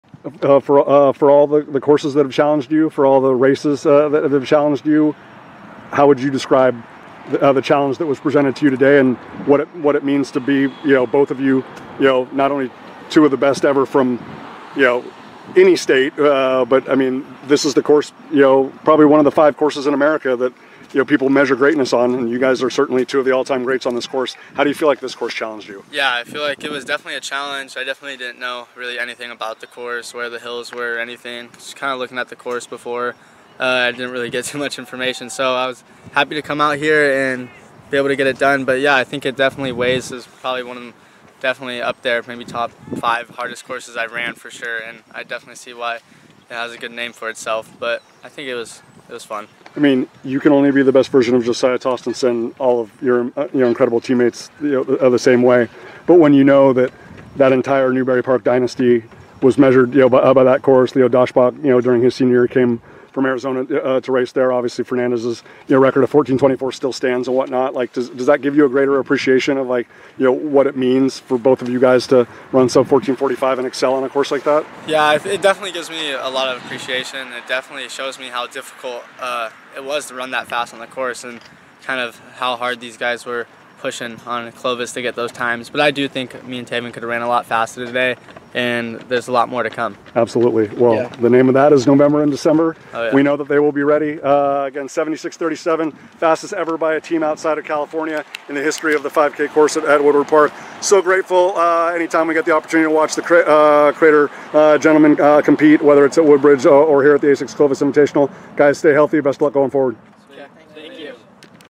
Josiah Tostenson of Crater 1st Place Boys 5K Rob Brenner Championship at ASICS Clovis Invitational
Josiah Tostenson, a senior at Crater High in Oregon, won the Rob Brenner boys championship race at the 45th ASICS Clovis Invitational by running 14:41.3 in his first career appearance on the 5-kilometer course at Woodward Park in Fresno, Calif. Tostenson elevated to the No. 13 all-time prep competitor at Woodward Park, helping the Comets become the fastest program outside of California ever to race on the 5-kilometer layout with a team time of 76:37, eclipsing the 1993 performance of 77:22 by Mead High from Washington at the Foot Locker West Regional. Crater ascended to the No. 6 all-time school at Woodward Park, producing the 12th-fastest overall performance, to take third with 116 points.